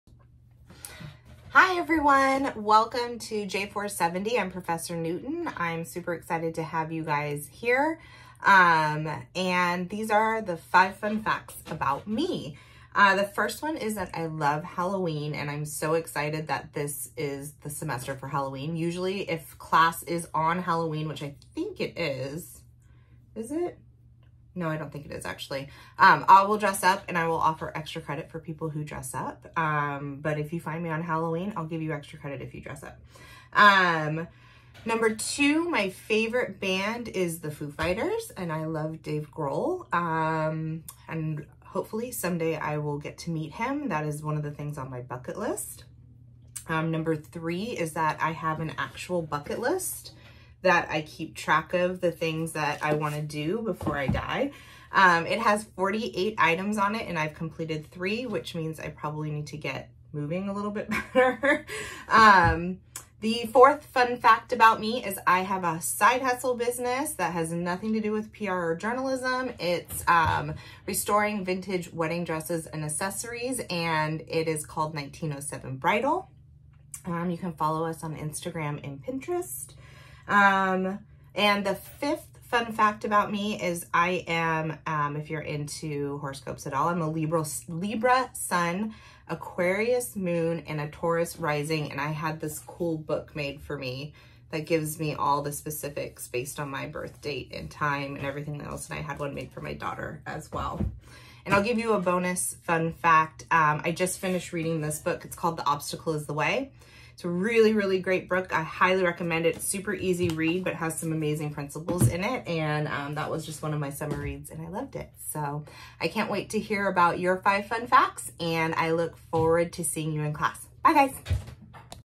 J470 Five Fun Facts
These are Prof. Newton’s Five Fun Facts for Fall 2022. Enjoy!

Also I really need to work on looking into the camera. Ugh.